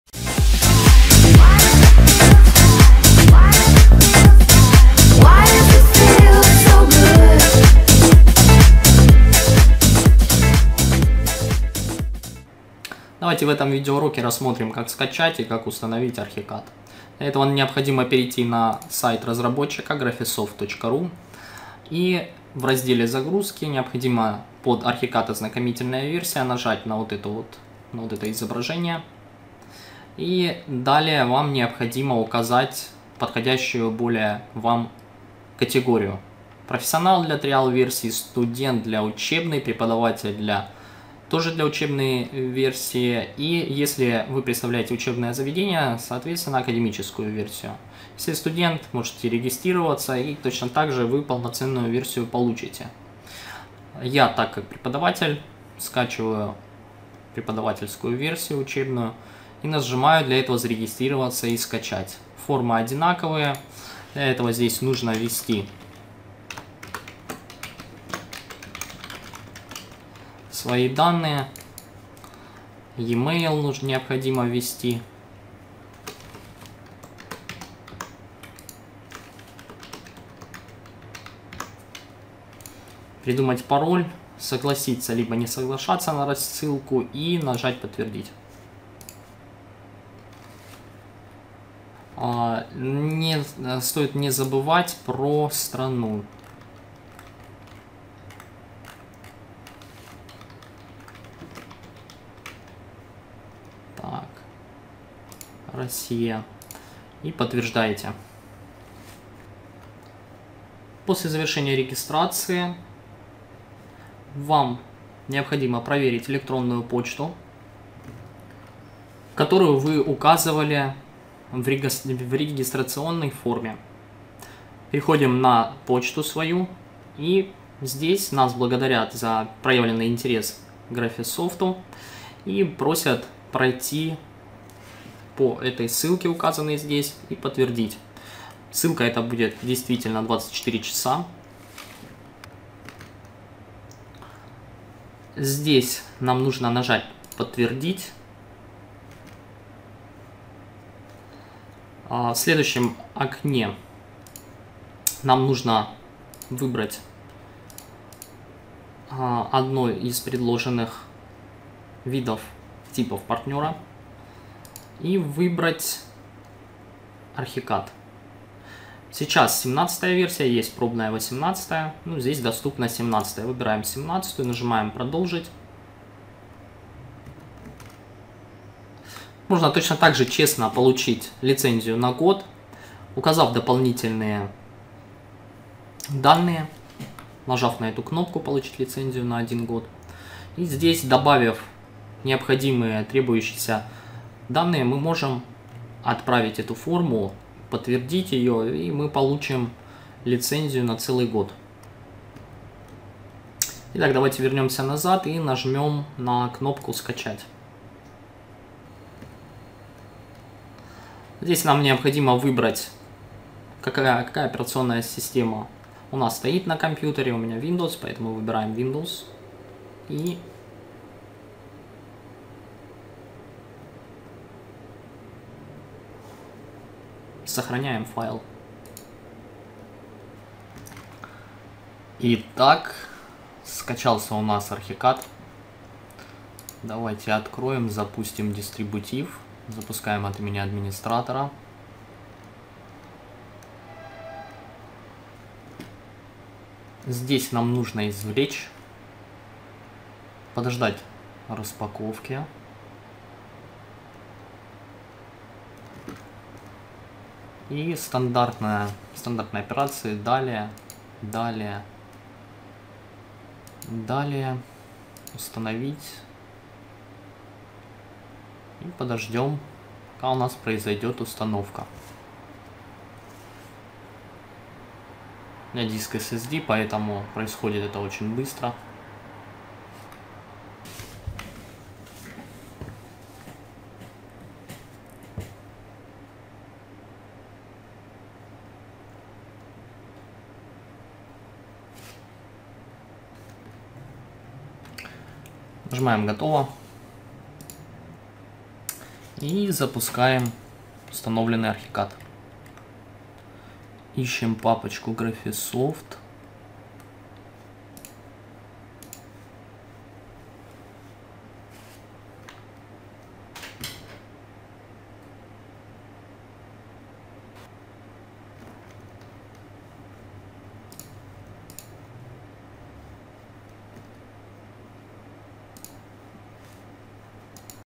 Как скачать и установить архикад
Рассмотрим как скачать и установить архикад